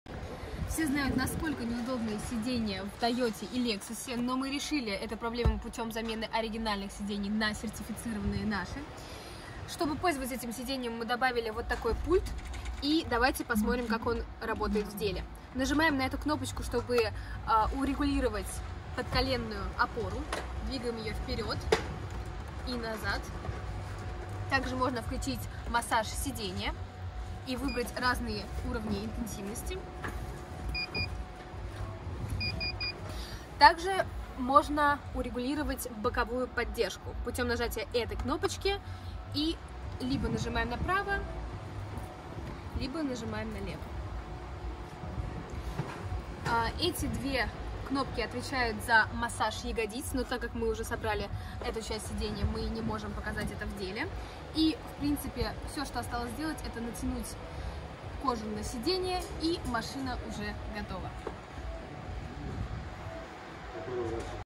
Комфортные сиденья LC200, LX450D/570 и G-Class 2019+
устанавливаем комфортные сиденья для новых автомобилей:
Mercedes G-Class 2019+
Lexus LX 570 / 450D
Toyota LC200

шикарные удобные сиденья как для водителя, так и для пассажиров